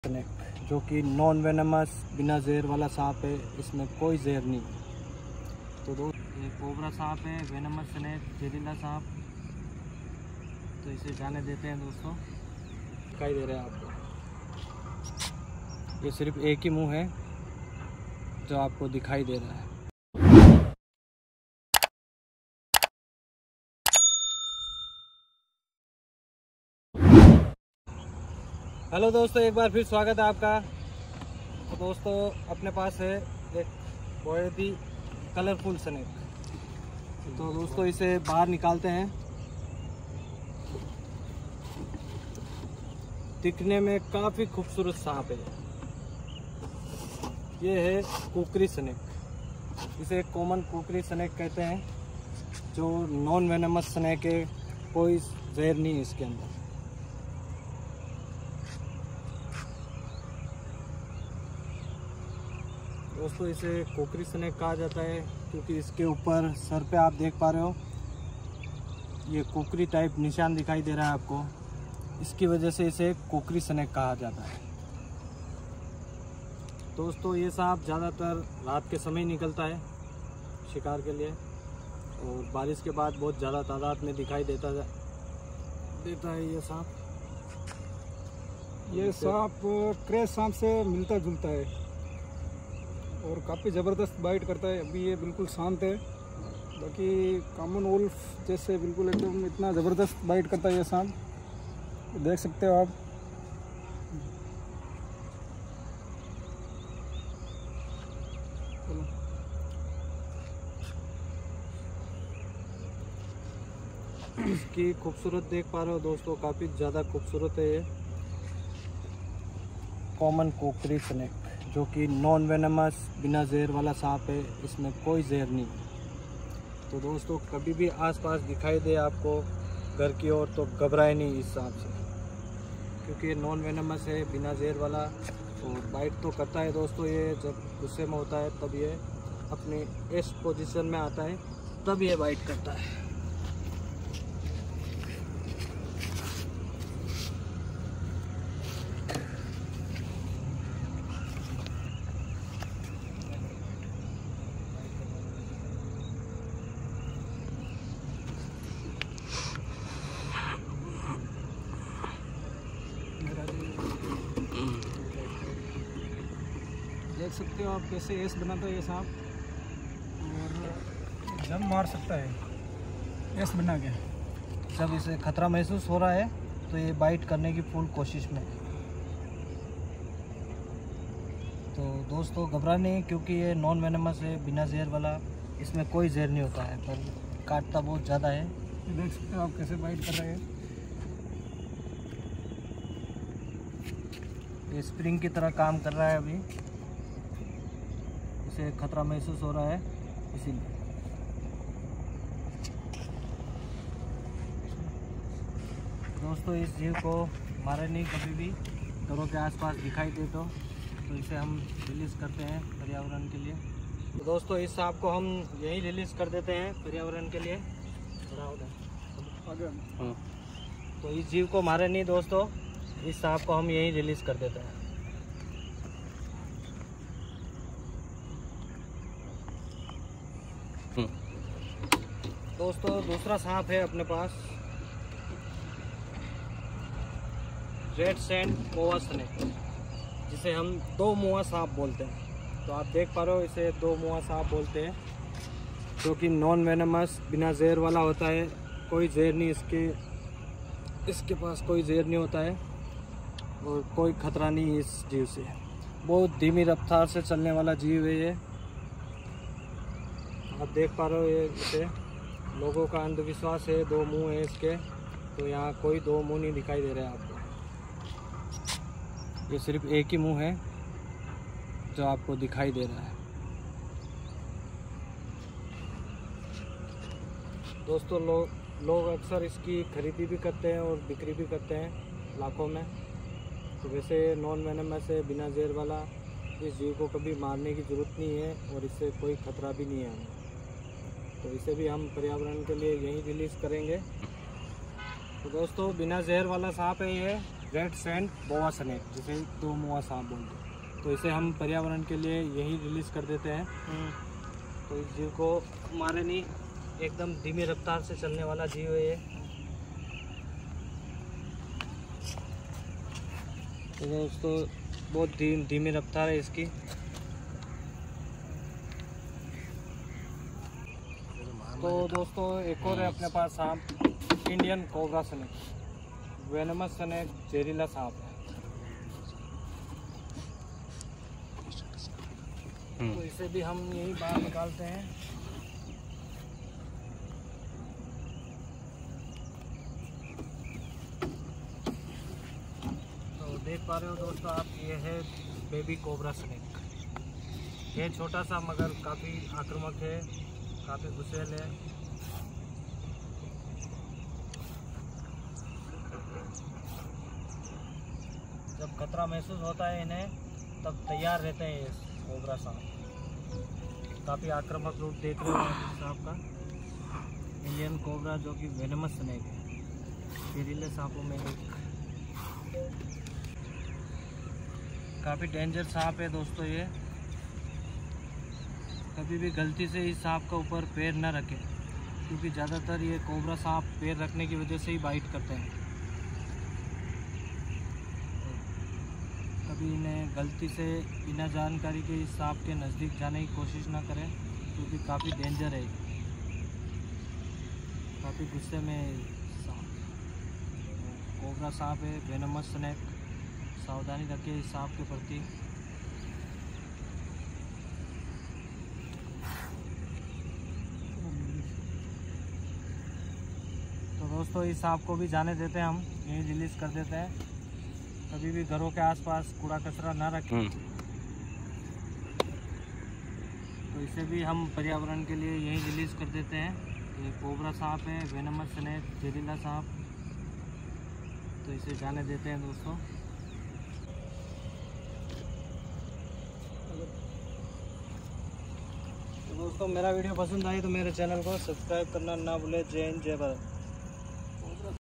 तीन प्रजाति के सांप release three snakes in forest safely ￼Kukri snake rad send boa snake cobrasnake
कलरफुल स्नैक जो बारिश के बाद दिखाई देता है इतनी धीमी रफ़्तार से चलने वाला सांप ज़हरीले सांपों में एक इंडिया कोबरा सांप तीनों सांपों को सुरक्षित जंगल में किया आजाद

Venomous Indian cobra snake

Read send Boa snake non venomous

Kukri snake non venomous